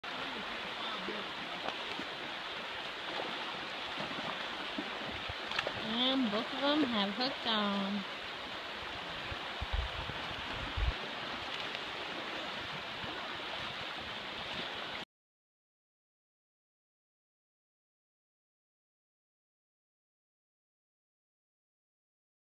Triple hook up fishing sockeyes at the Russian River Alaska
Sockeye salmon fishing triple hook up